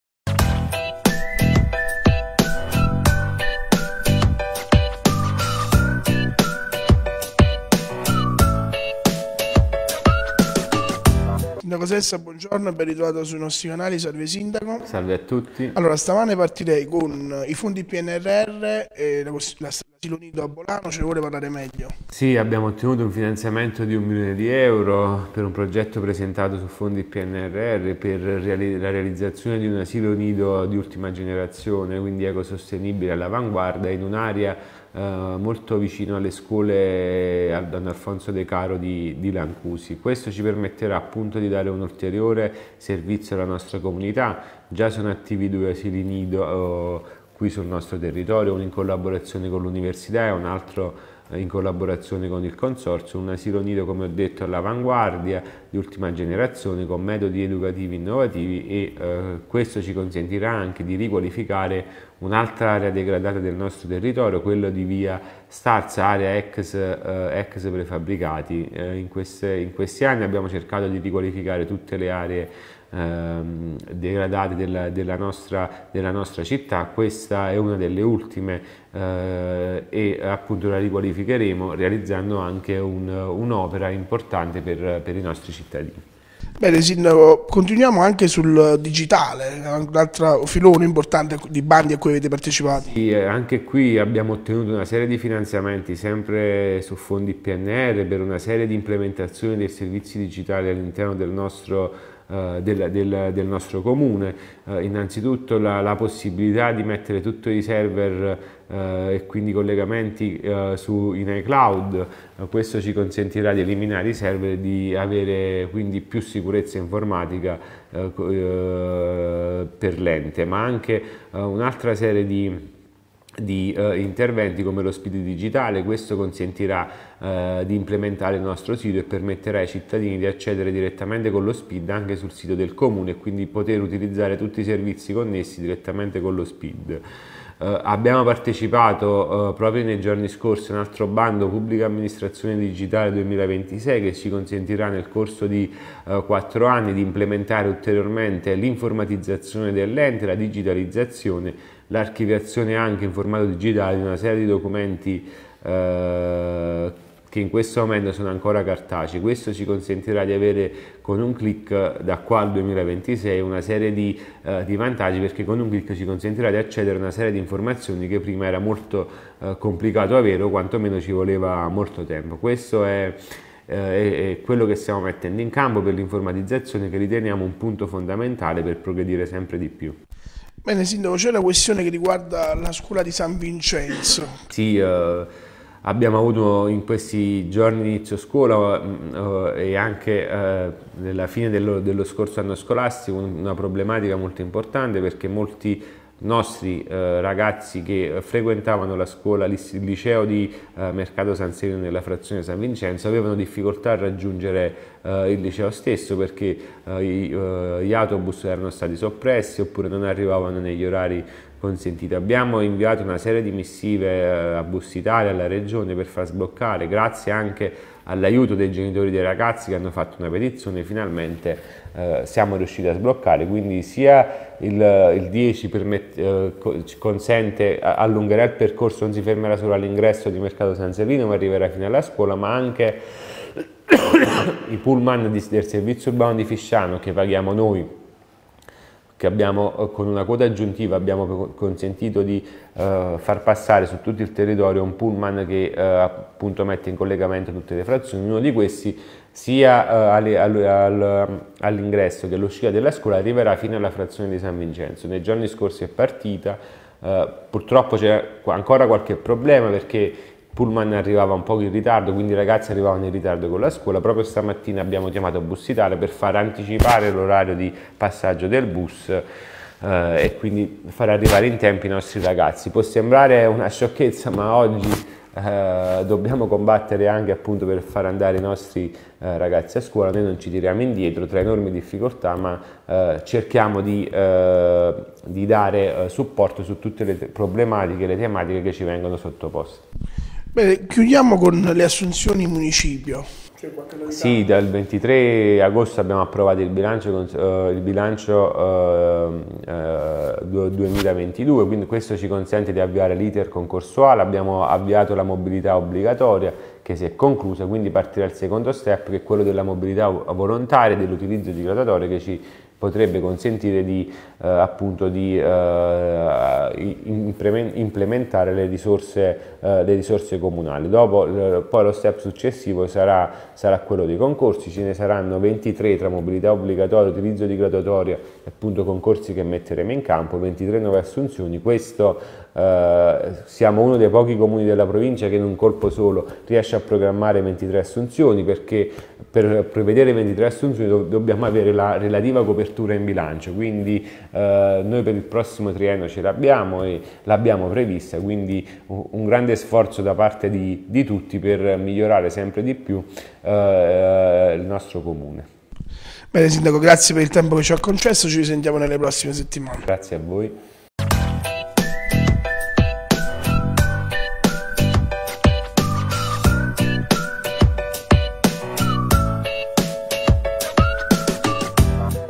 Fisciano (SA) - Asilo nido a Bolano, Pa digitale 2026 (21.09.22)
- Fisciano (SA) - Asilo nido a Bolano, Pa digitale 2026
Asilo nido a Bolano, Pa digitale 2026, questione bus San Vincenzo, Assunzioni in Municipio: l'intervista al Sindaco di Fisciano Vincenzo Sessa.  (21.09.22)

Video ZON Zerottonove